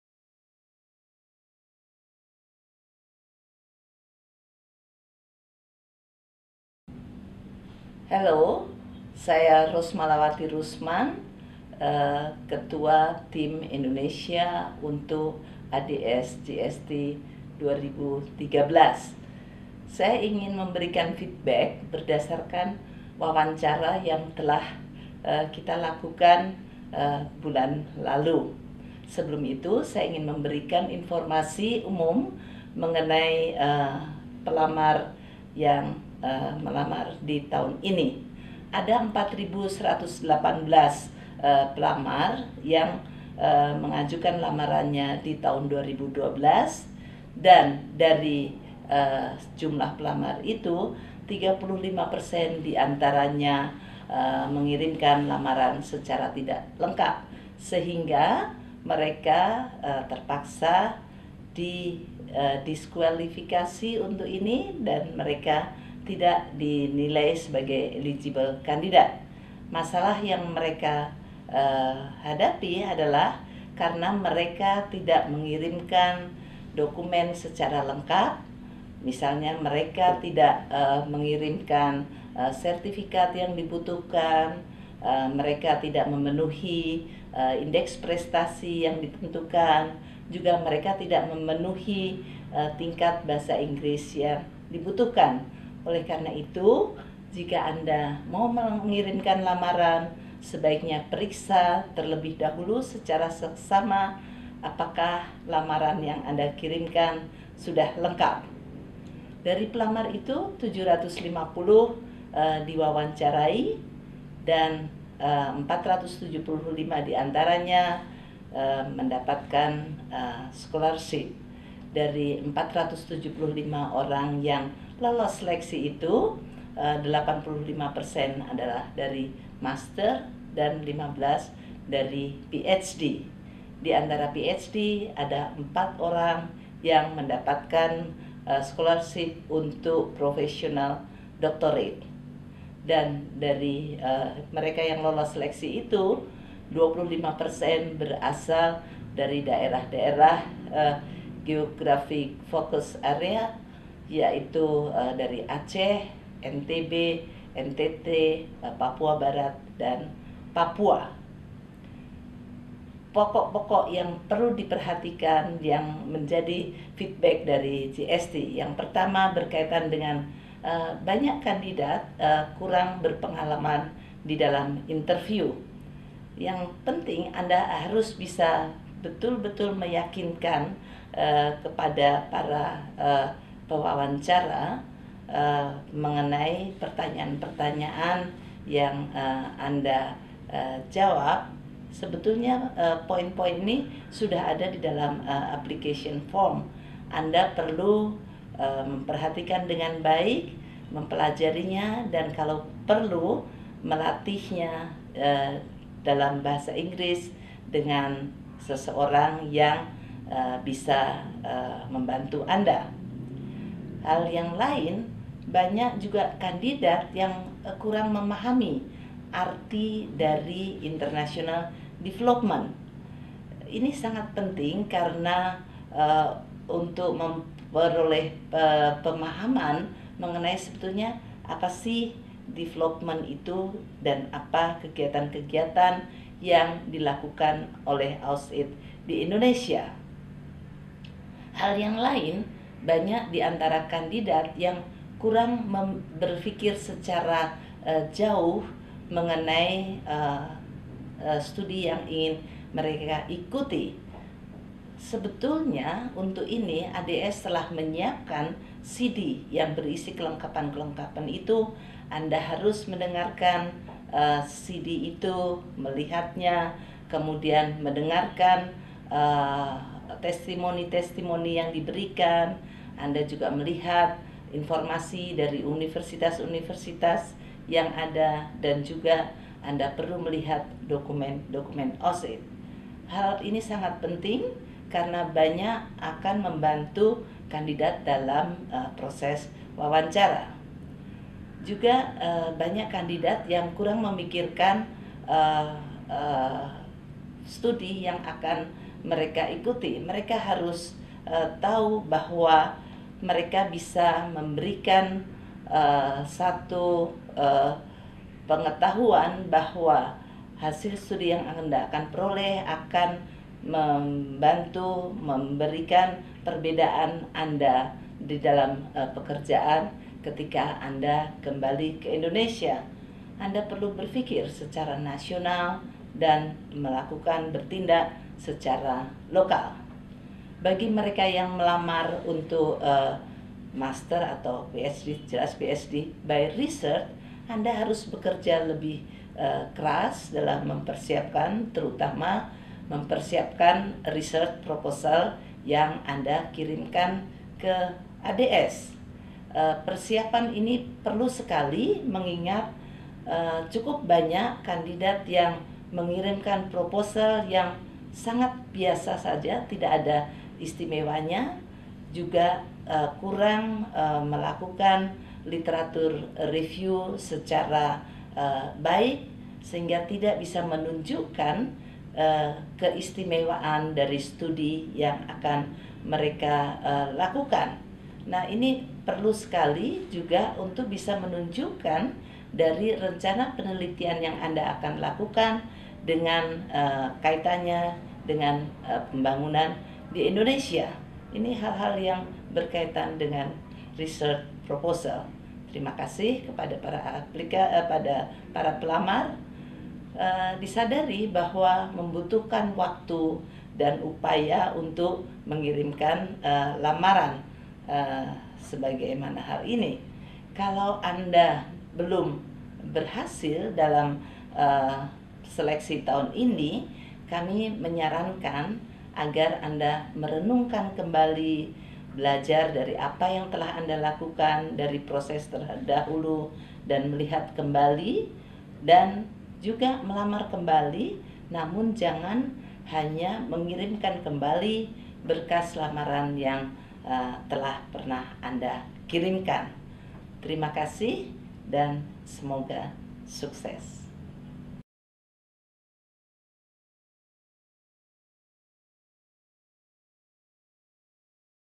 Feedback from the Joint Selection Team Interviews
Video ini diambil dari Tips ini disampaikan langsung oleh ketua tim seleksi AAS/ADS Indonesia sehingga isinya terpercaya. Pejuang beasiswa AAS/ADS wajib menyimak ini.